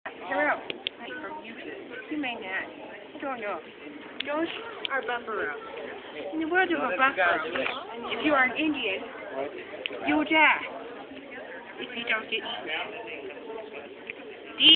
Buffalo And Indian Food Chain
At and Indian exibet and Ralex explains the Indian Life